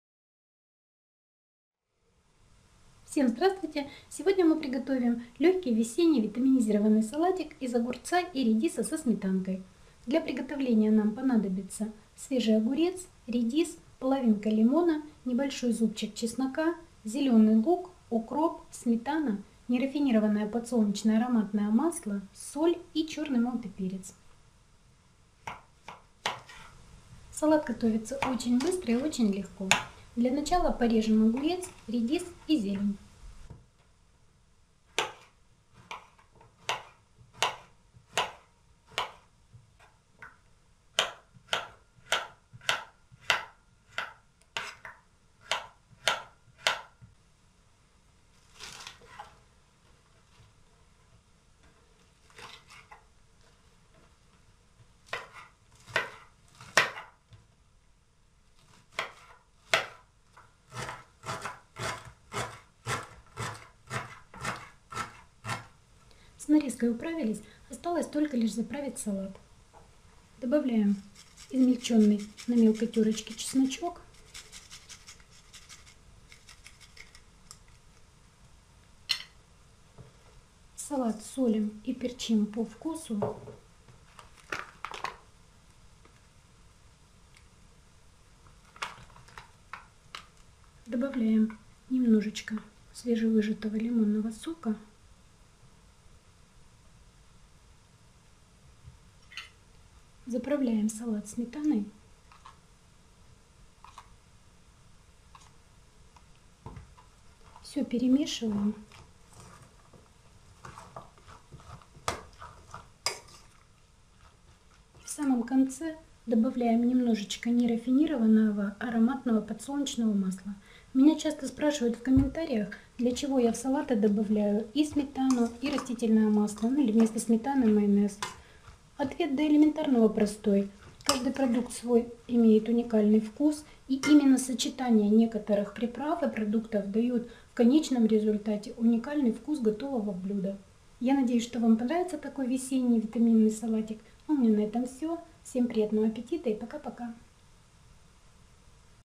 Салат из огурца и редиса. Salad cucumber and radish.
Готовим весенний салат из огурца и редиса со сметаной.